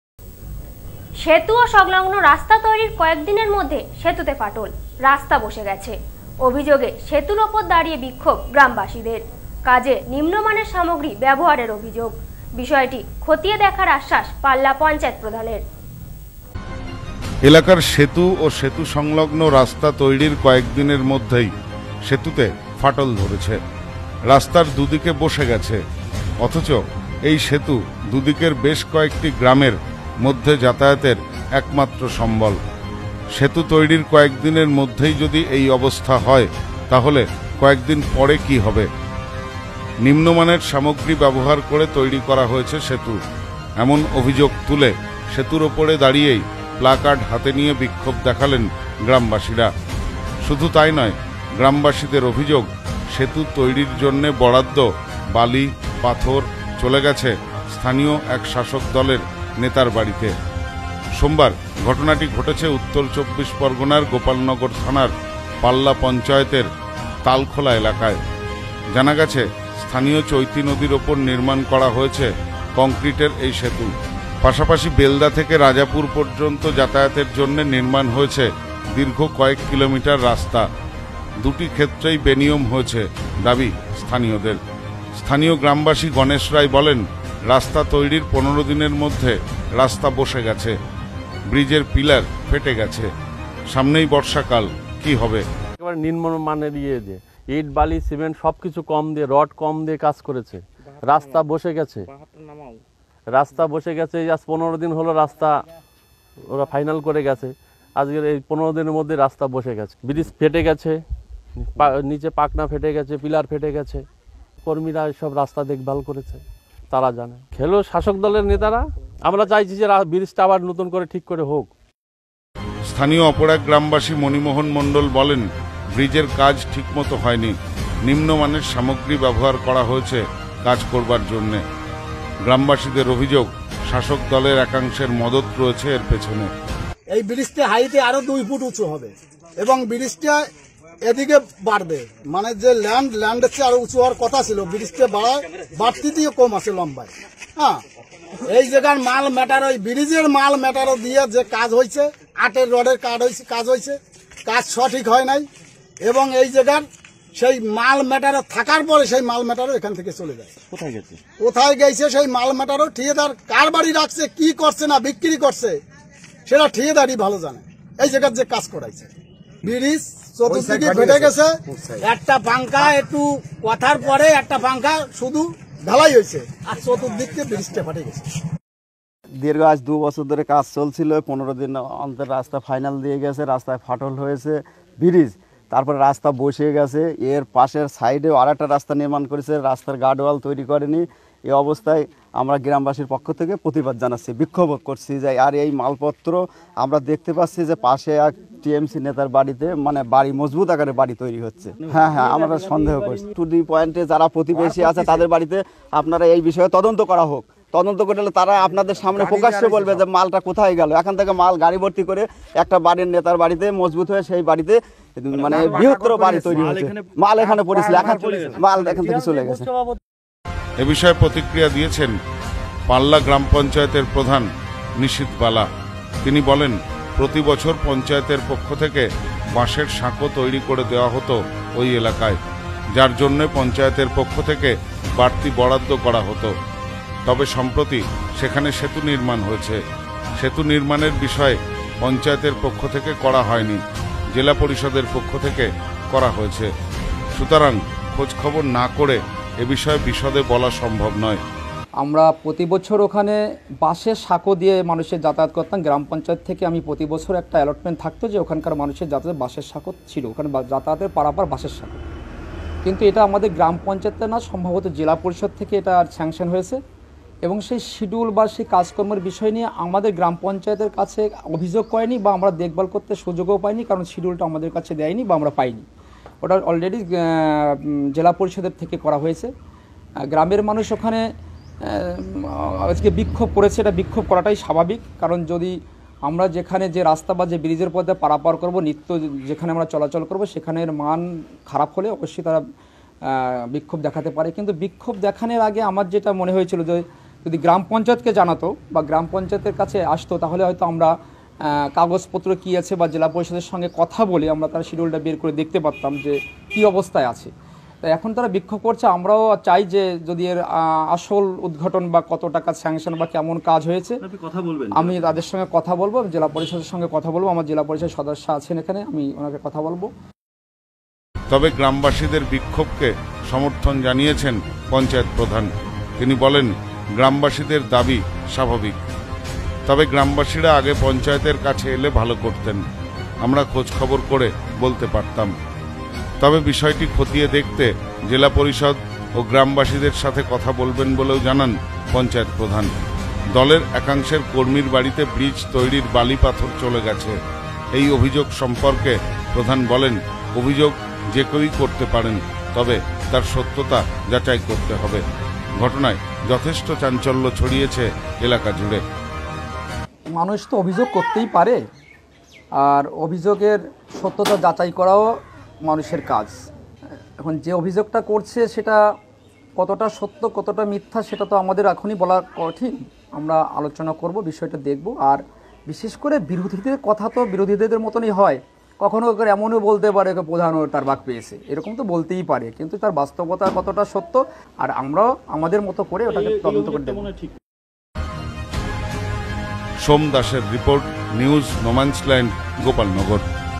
সেতু ও সংলগ্ন রাস্তা তৈরির কয়েক দিনের মধ্যে সেতুতে ফাটল।
সেতু ও সংলগ্ন রাস্তা তৈরির কয়েক দিনের মধ্যে সেতুতে ফাটল। অভিযোগে সেতুর উপর দাঁড়িয়ে বিক্ষোভ গ্রামবাসীদের। কাজে নিম্নমানের সামগ্রী ব্যবহারের অভিযোগ। বিষয়টি খতিয়ে দেখার আশ্বাস পাল্লা পঞ্চায়েত প্রধানের।